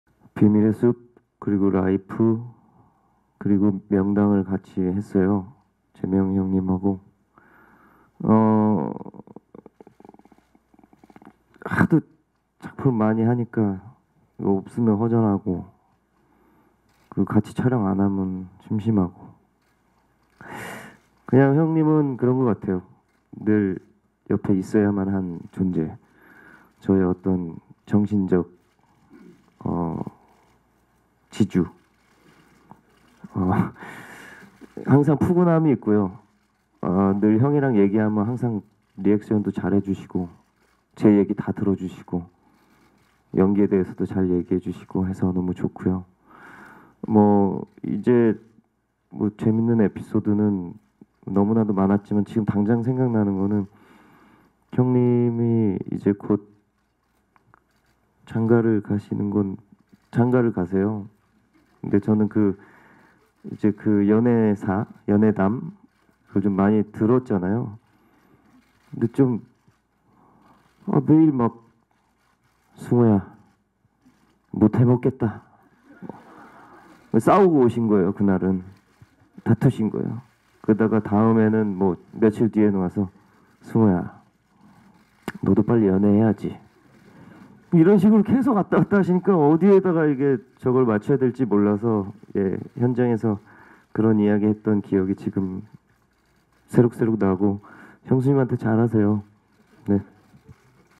[영상] 조승우(Jo Seung Woo) “결혼 앞둔 유재명, ‘너도 연애 해야지’라고 말하면…” (명당)
[뉴스인사이드 조성욱 기자] 배우 조승우가 유재명과의 인연을 언급했다.

11일 오후 서울 강남구 메가박스 코엑스점에서 열린 영화 ‘명당’ 언론시사회에 박희곤 감독을 비롯해 배우 조승우, 지성, 백윤식, 김성균, 유재명, 이원근이 참석했다.

이날 조승우는 “유재명과 ‘비밀의 숲’ ‘라이프’ ‘명당’을 함께 했다. 같이 작품을 많이 하니까 없으면 허전하고 같이 촬영 안하면 심심하다. 형님은 늘 옆에 있어야만 하는 존재고, 정신적 지주다. 형이랑 이야기 하면 제 얘기도 다 들어주고 연기에 대해서도 잘 얘기해줘 너무 좋다”고 전했다.

이어 “형님이 곧 장가를 가신다. 그 연애담을 많이 들었다”며 “‘승우야 못해먹겠다’라고 하면 그 날은 싸우고 오신거다. 다음에는 ‘승우야 너도 연애 해야지’라고 하더라. 어디에 장단을 맞춰야 할지 모르겠더라. 형수님한테 잘해주면 좋겠다”고 웃으며 말했다.

영화 ‘명당’은 땅의 기운을 점쳐 인간의 운명을 바꿀 수 있는 천재 지관 박재상과 왕이 될 수 있는 천하명당을 차지하려는 이들의 대립과 욕망을 그린 작품. 오는 19일 개봉한다.